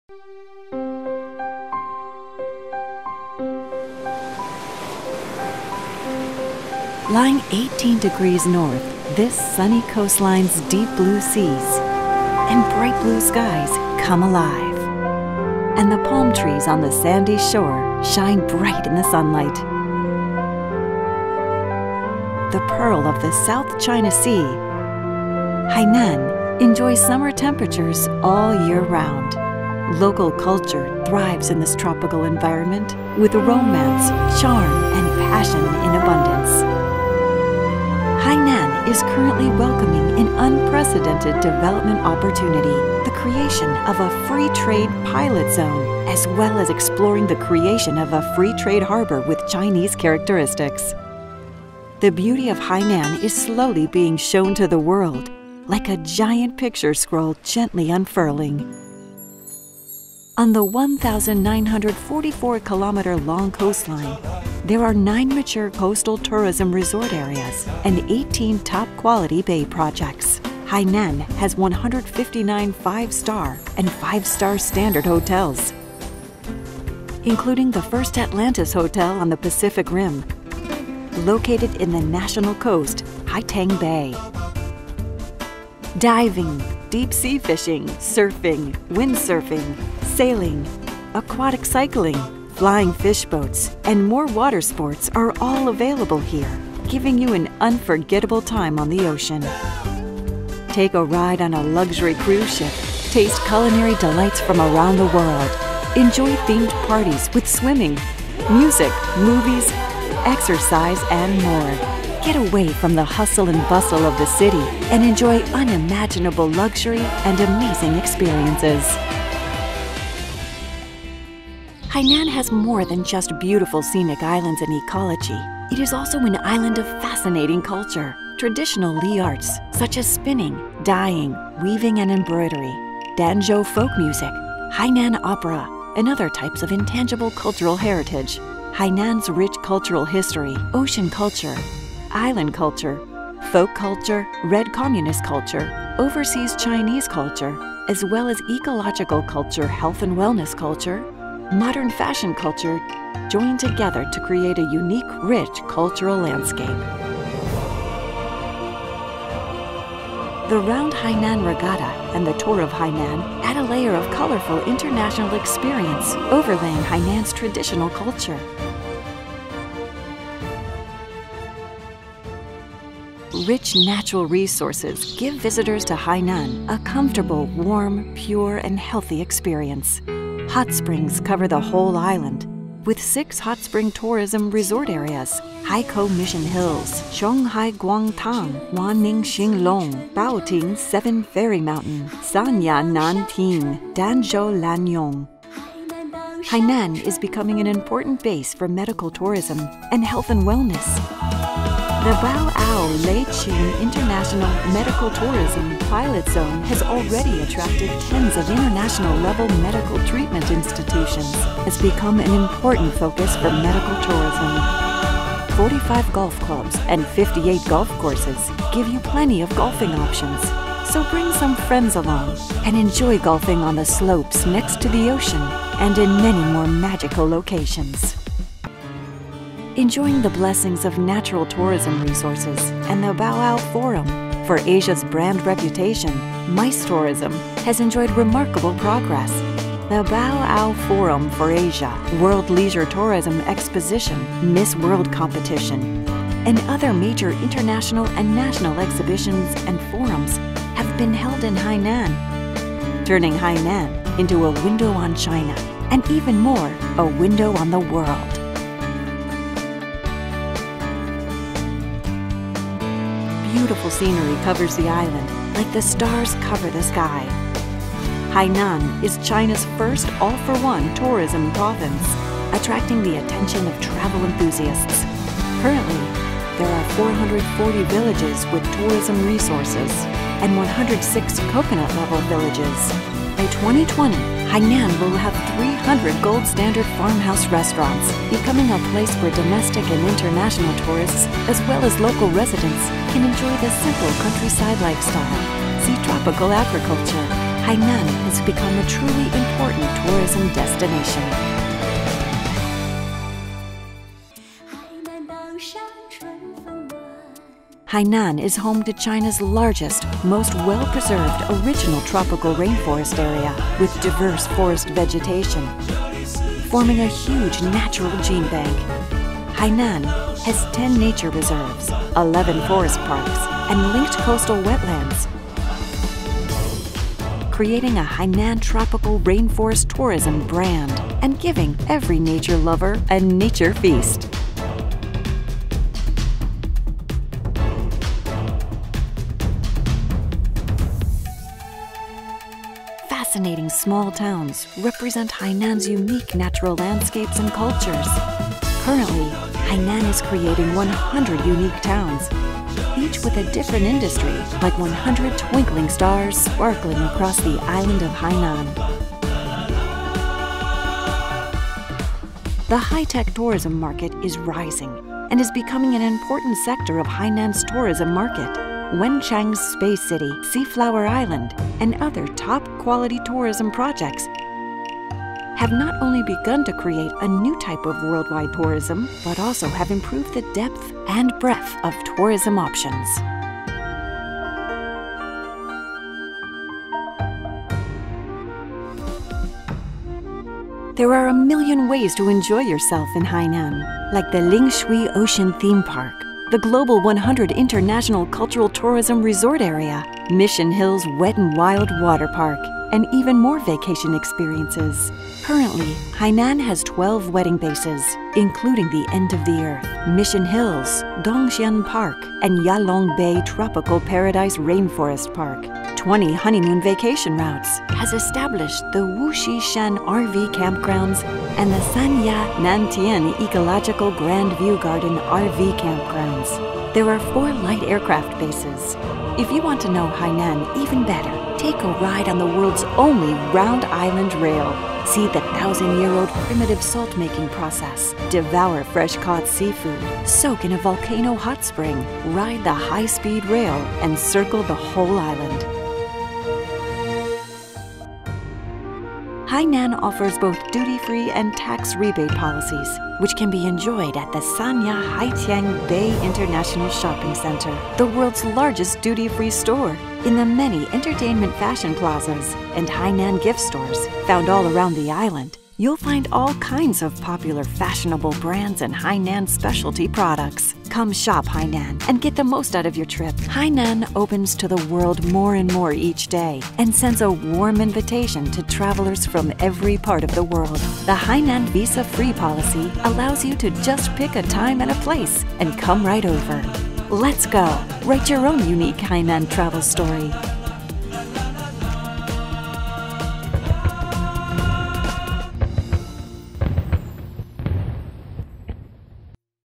Hainan - The Beautiful Tropical Island
Hainan is the southernmost province of China, which is located at north latitude 18°10’～20°10’ and east longitude 108°37’～111°03’. The province covers a land area of 35,400 square kilometers and a sea area of approximately 2 million square kilometers. The coastline is 1944 kilometers long and has 68 large and small harbors. The contours of Hainan Island are like an oval large Sydney. It is the only tropical island province in China and the most popular hot island resort, and has the reputation of "Oriental Hawaii".

Hainan province is called “Qiong” for short and the capital of the province is Haikou. There are 27 cities, counties (districts) in the province. By the end of 2017, the province has population size of 9,257,600, belonging to more than 30 ethnic groups including Han, Li, Miao and Hui. Among them, the Li nationality is an aboriginal ethnic group on Hainan Island, and the unique ethnic customs makes the society of Hainan more colorful. The main dialects spoken in Hainan are Hainan dialect, Li dialect, Lingao dialect, Danzhou dialect, Jun dialect, Miao dialect, etc. Among them, the Hainan dialect is the most widely spoken and most used in everyday life.
The forest coverage rate of Hainan Province is 62.1%. There are 81 mountain peaks over a kilometer on the island, 10 National Forest Parks, 22 Provincial Forest Parks, and 4 National Wetland Parks. The province has established 49 nature reserves for ecosystems, wild animals and plants, and natural landscapes, with a total area of 27,023 square kilometers. There are 18 species of first-class protected animals, including Hainan sloping deer, Hainan black-crested gibbon, clouded leopard, monitor lizard, and Hainan partridge.

Being the pearl of the South China Sea, Hainan enjoys summer temperatures all year round. Local culture thrives in this tropical environment, with romance, charm, and passion in abundance. Hainan is currently welcoming an unprecedented development opportunity – the creation of a free trade pilot zone, as well as exploring the creation of a free trade harbor with Chinese characteristics. The beauty of Hainan is slowly being shown to the world, like a giant picture scroll gently unfurling.

The video is provided by the Department of Culture Radio Television Publication and Sports of Hainan

YOUTUBE: China Cultural Center in Copenhagen